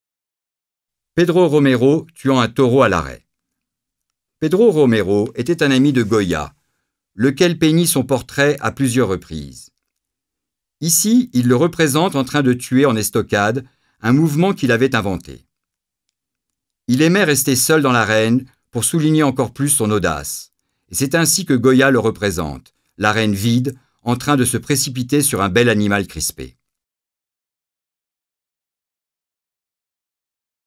Pedro Romero tuant un taureau à l’arrêt. Francisco de Goya
Pedro Romero était un ami de Goya, lequel peignit son portrait à plusieurs reprises. Ici, il le représente en train de tuer en estocade, un mouvement qu’il avait inventé. Il aimait rester seul dans l’arène pour souligner encore plus son audace, et c’est ainsi que Goya le représente, l’arène vide, en train de se précipiter sur un bel animal crispé.

Goya vit à Saragosse! Visitez-le dans le nouveau musée Goya. Collection Ibercaja - Musée Camón Aznar.